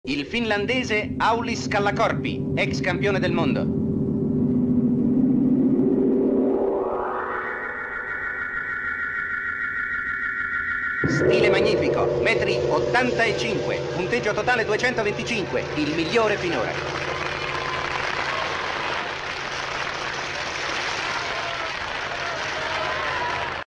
AULIS KALLAKORPI Cortina d’Ampezzo 1956 Skijumping Skispringen
Aulis Kallakorpi (1 January 1929 in Kuusankoski – 15 May 2005 in Mikkeli) was a Finnish ski jumper. He won a silver medal in the Individual large hill event at the 1956 Winter Olympics.

Kallakorpi also won the ski jumping event at the Holmenkollen ski festival in 1955.

Kallakorpi oli mäkihypyn huippunimiä 1950-luvulla. Hänen parhaimpiin saavutuksiinsa kuuluvat vuoden 1956 talviolympialaisten hopeamitali, Holmenkollenin mäkikisan voitto ensimmäisenä suomalaisena ja toisena ei-norjalaisena 1955[2] ja Salpausselän mäkikisan voitto vuonna 1953. Keski-Euroopan mäkiviikolla hän voitti Oberstdorfin ja Garmisch-Partenkirchenin osakilpailut kaudella 1954–1955 sekä jakoi Oberstdorfin osakilpailun voiton Eino Kirjosen kanssa kaudella 1955–1956. Mäkiviikon kokonaiskilpailussa hän oli kolmas kaudella 1954–1955. SM-kisoissa Kallakorpi saavutti hopeaa 1952.